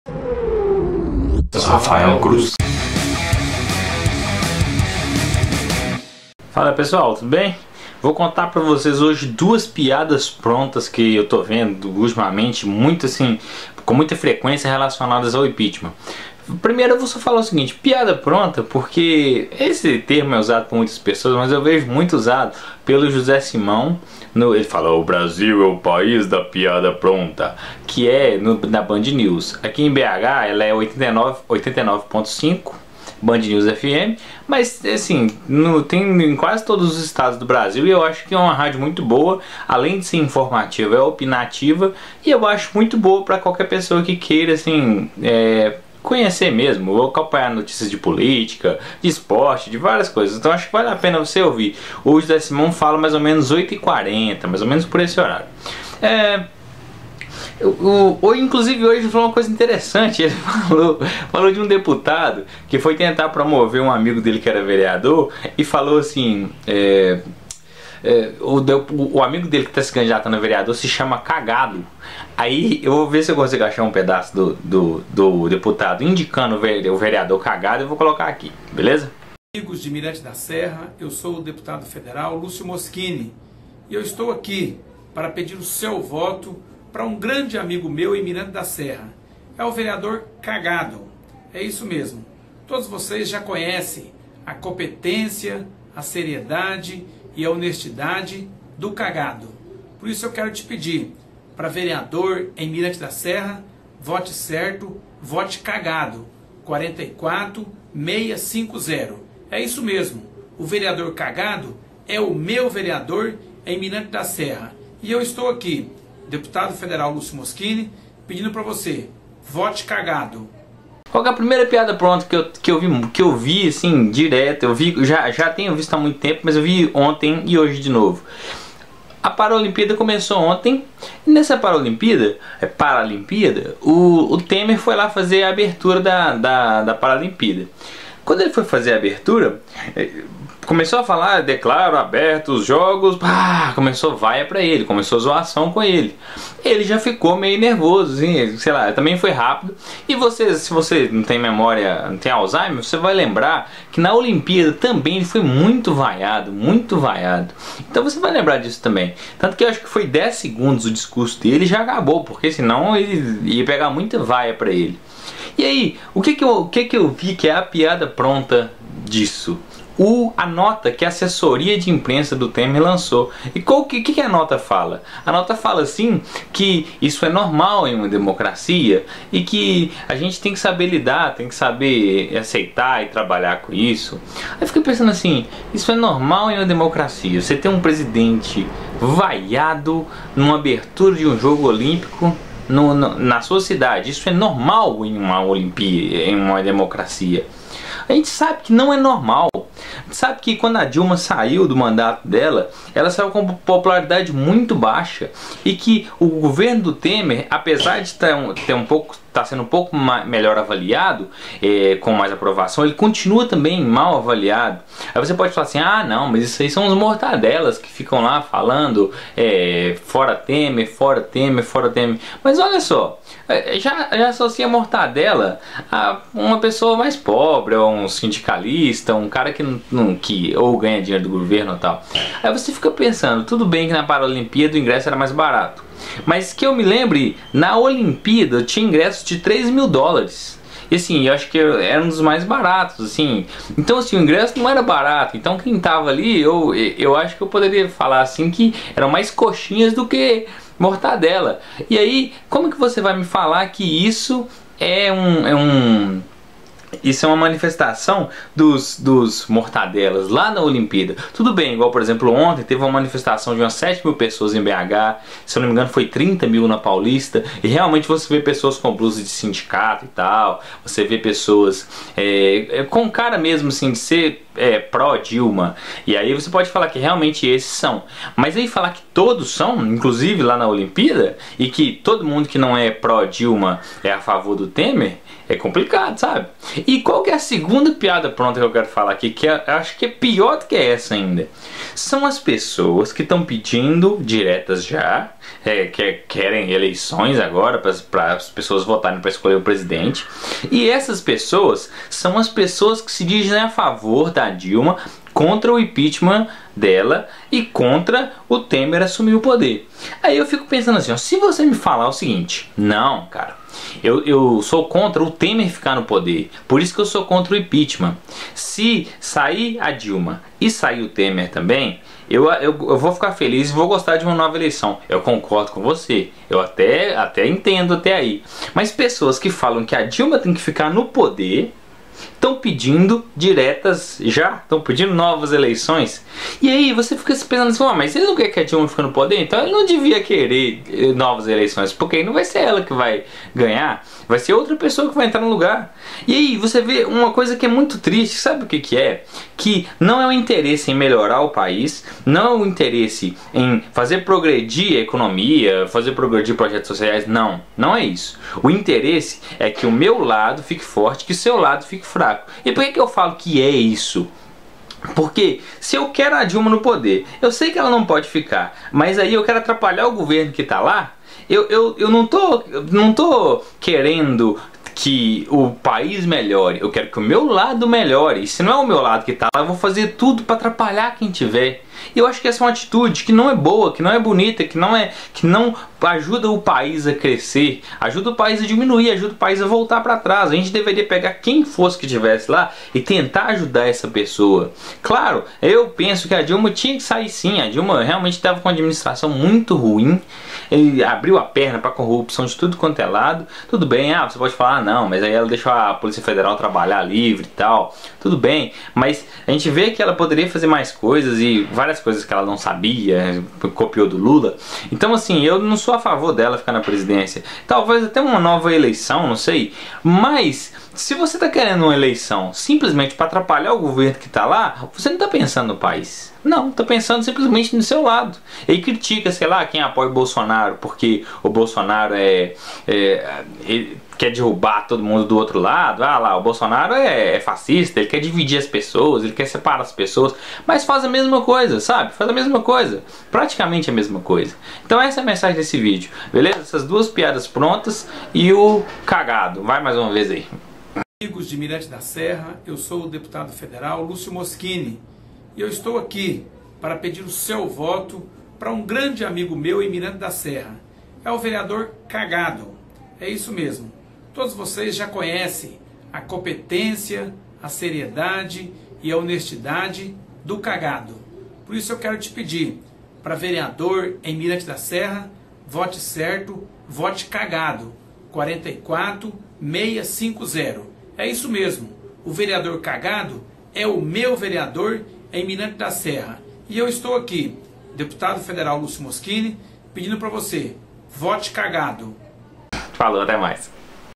FORA TEMER é algo natural na DEMOCRACIA#SQN
Nesse vídeo falo sobre o clamor do Fora Temer e dos pedidos sobre eleições diretas.
Além disso comento sobre as pessoas que pedem eleições diretas e ainda dizem apoiar a Dilma.